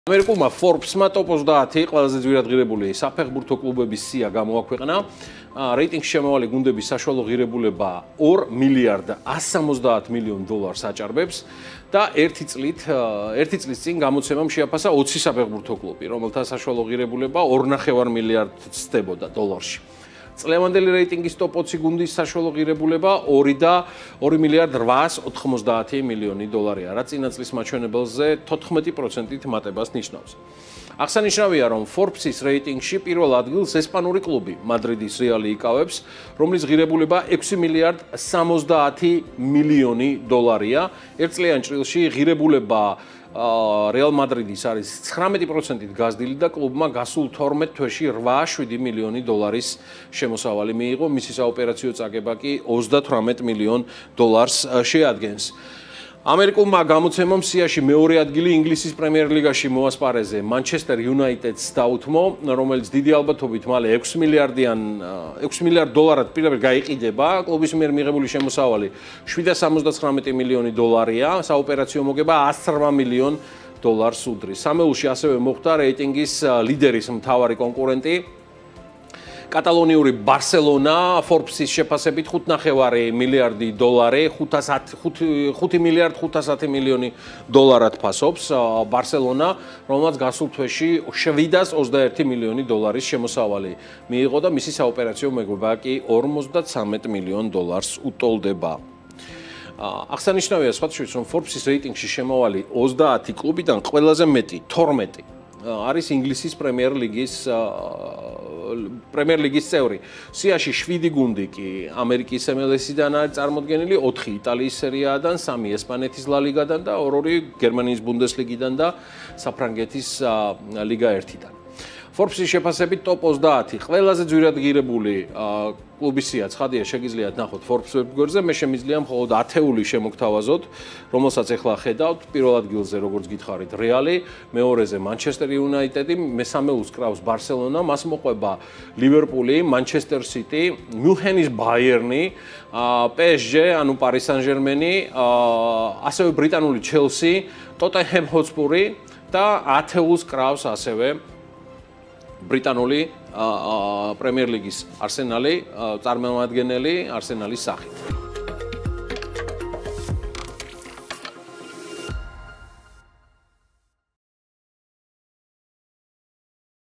FORBESკვირა - რეიტინგი - ყველაზე ძვირადღირებული საფეხბურთო კლუბები
Forbes-ის რეიტინგში შემავალი 30 კლუბიდან ყველაზე მეტი, 12, ინგლისის პრემიერ-ლიგაში ასპარეზობს. სიაში შვიდი გუნდი ამერიკის MLS-დანაა წარმოდგენილი, ოთხი – იტალიის სერია A-დან, სამი – ესპანეთის ლა ლიგიდან, ორ-ორი კი – გერმანიის ბუნდესლიგიდან და საფრანგეთის ლიგა 1-დან.